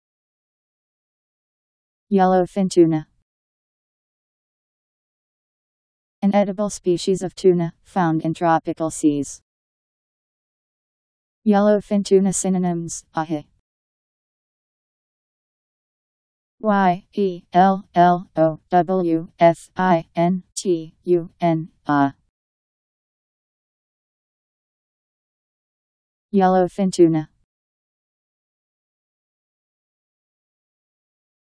Yellowfin tuna Meaning
Video shows what yellowfin tuna means. An edible species of tuna, , found in tropical seas.. yellowfin tuna synonyms: ahi.  Yellowfin tuna Meaning. How to pronounce, definition audio dictionary. How to say yellowfin tuna. Powered by MaryTTS, Wiktionary